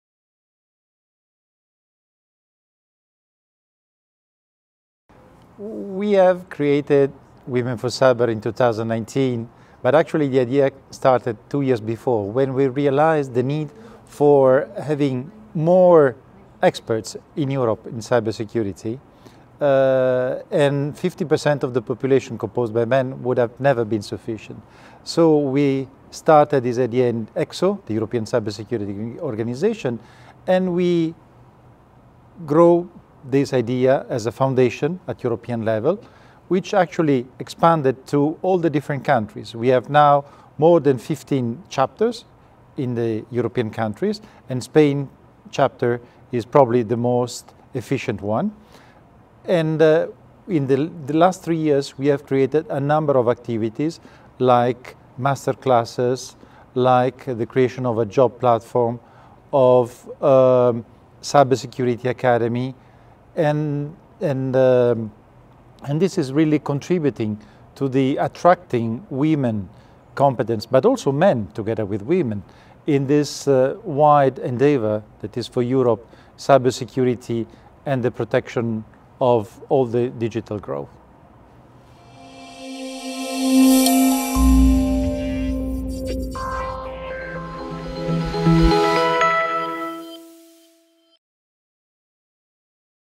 WOMEN4CYBER with Luigi Rebuffi
WOMEN4CYBER: new initiatives in Cibersecurity led by women.